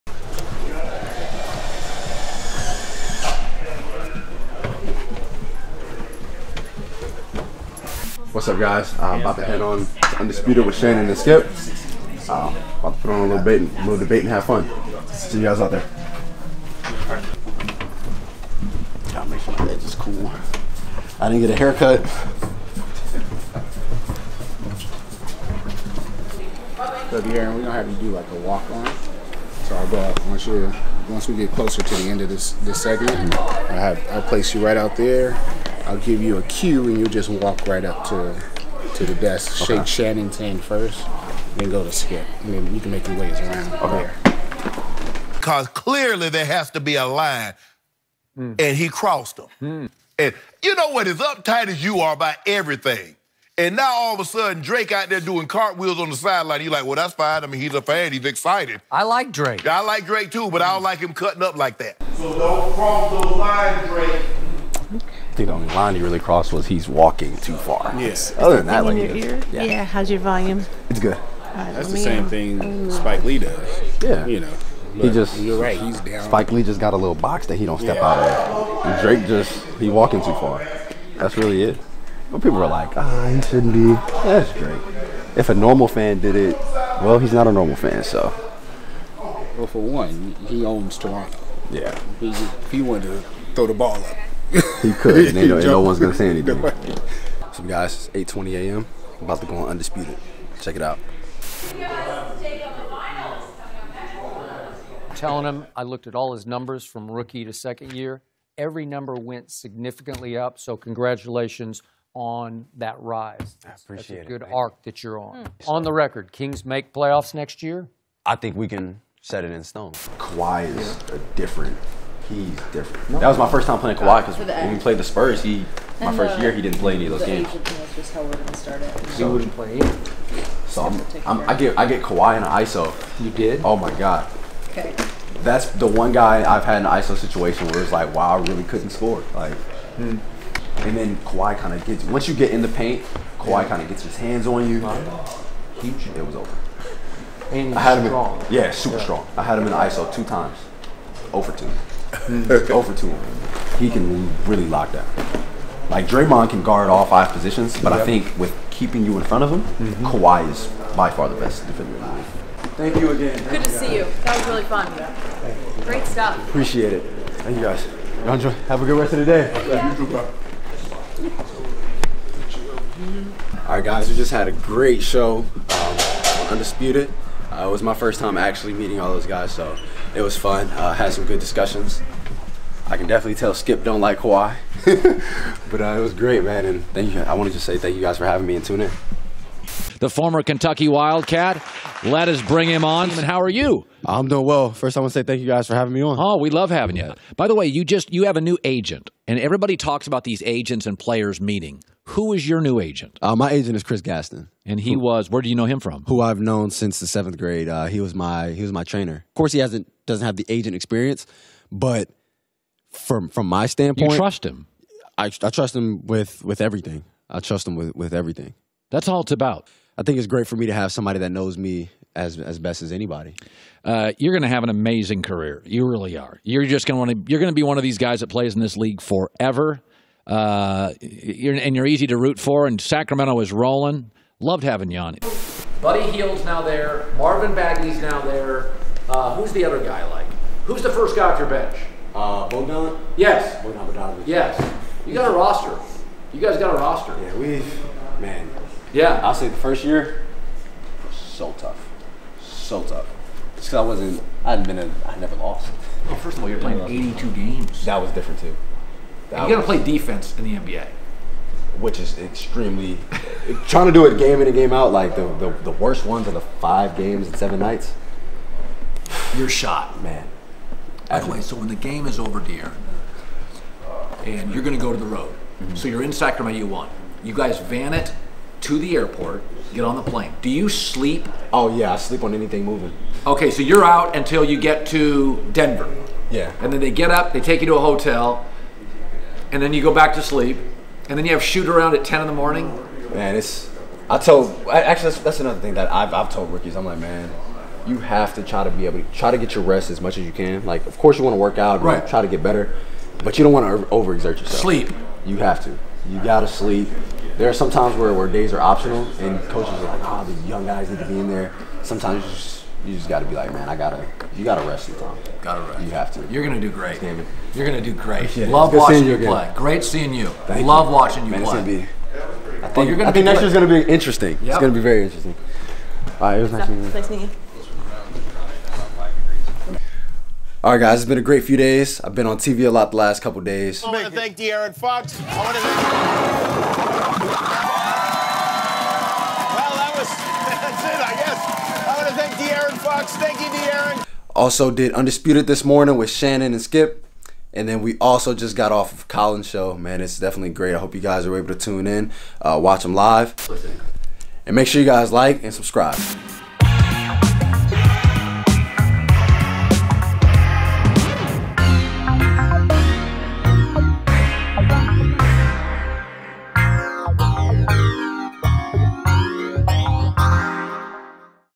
De'Aaron Fox Foxin Around Fox Sports Vlog
Check it out I'm bringing you guys a full behind the scenes video from my day in Los Angeles, while I went and stopped by The Herd and Undisputed on their sets. I enjoyed being on there sets to stop by and talk about this upcoming future and the sports now.